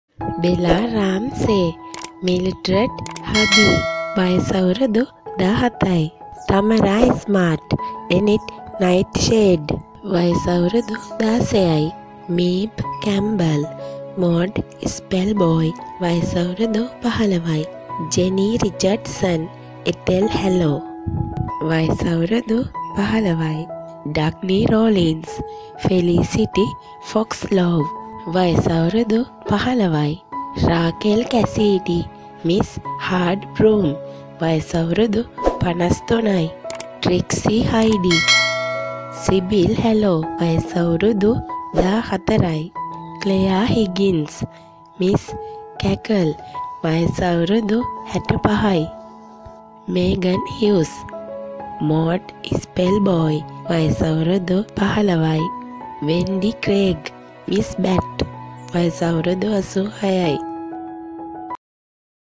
Maya Pasala Cast With Real Age And Name | The Worst Witch Cast
Maya Pasala Drama is beautiful drama aired on Tv derana .

I Thought You Might Want To Know The Real Names And Ages Of The Cast In This Drama .

1 . Bella Ramsey

2 . Tamara Smart

3 . Meibh Campbell

4 . Jenny Richardson

5 . Dagny Rollins

6 . Raquel Cassidy

7 . Trixie Hyde

8 . Clare Higgins

9 . Megen Hughes

10 . Wendy Craig

Then Watch The Video And Tell Me If Is A Good Or Bad .